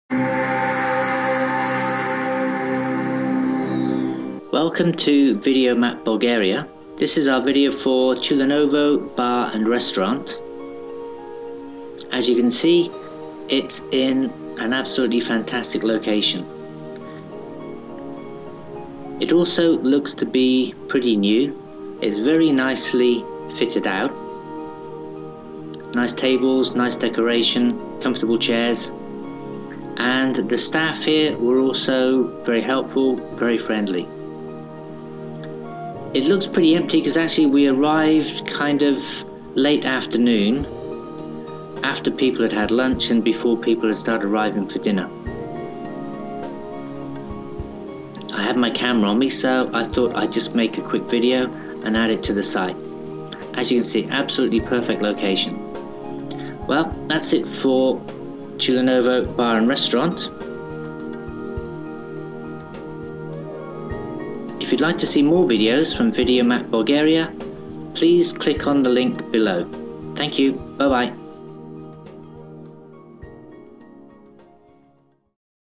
Tyulenovo restaurant - perfect location for dinner or a drink. Bulgaria.
Tyulenovo restaurant - perfect location for dinner or a drink. Bulgaria. Sitting right next to a pretty fishing cove, Tuyanovo restaurant has the absolutely perfect location. Friendly staff and good food also help.